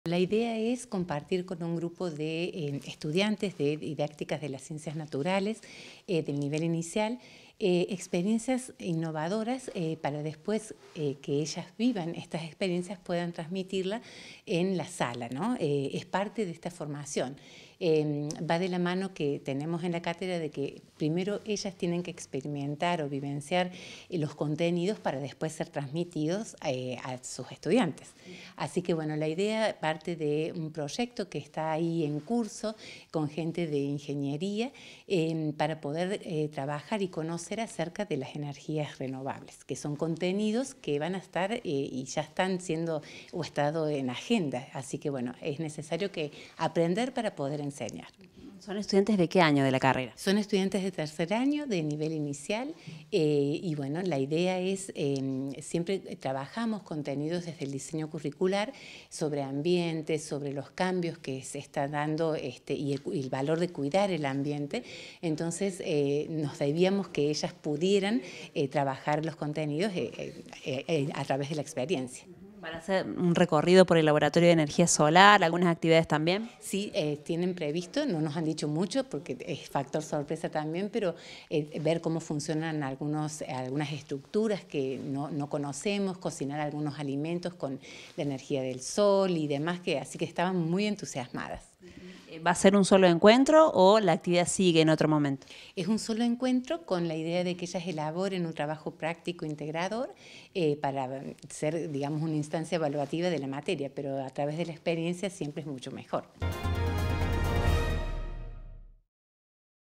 ESTUDIANTES DE 3º AÑO DE LA CARRERA DE EDUCACIÓN INICIAL VISITARON EL LABORATORIO DE ENERGÍA SOLAR
MARIA LAURA DE LA BARRERA – DOCENTE
UniRio TV
Universidad Nacional de Río Cuarto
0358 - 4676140